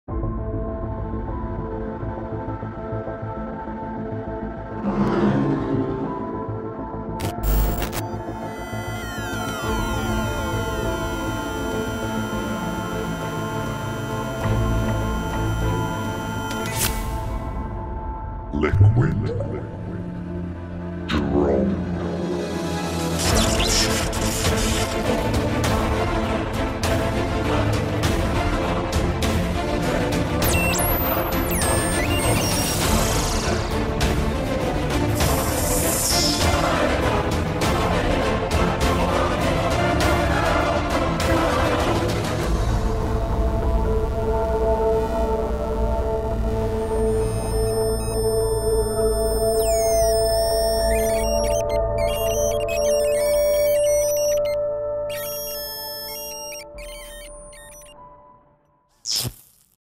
B ROLL with iPhone11 PRO max Screwed Circuitz PROMO video
Screwed Circuitz build unconventional electronic musical & noise instruments by hand! These products are available @moogaudioofficial